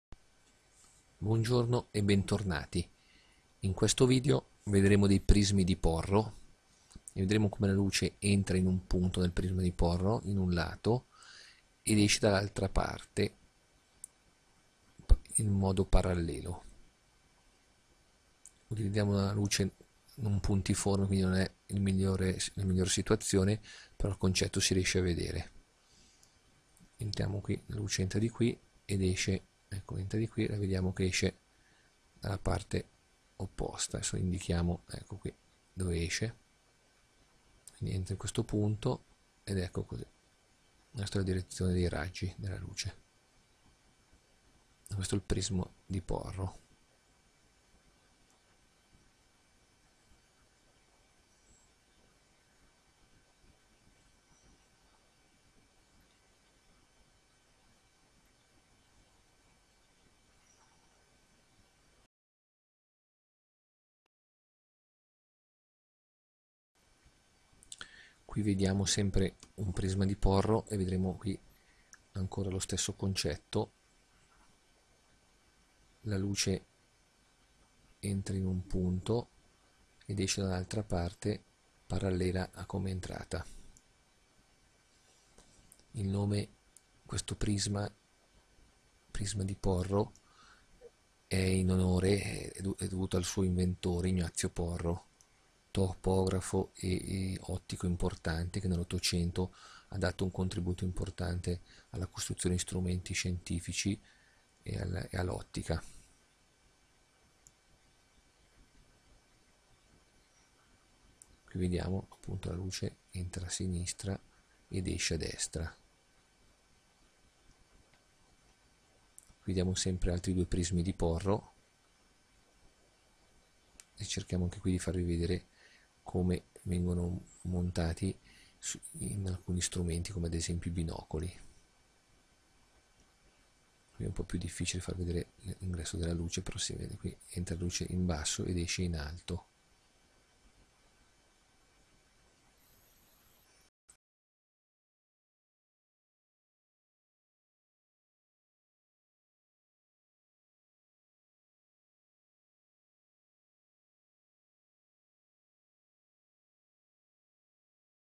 Prismi di Porro. Ottica geometrica pratica. I prismi in vetro ottico. Ottica pratica e utile
Prismi di Porro. Usati per raddrizzare un'immagine in molti strumenti come ad esempio nri i binocoli. Il nome è in onore dell'inventore Ignazio Porro, ottico e topografo che tanto ha contribuito al progresso dell'ottica.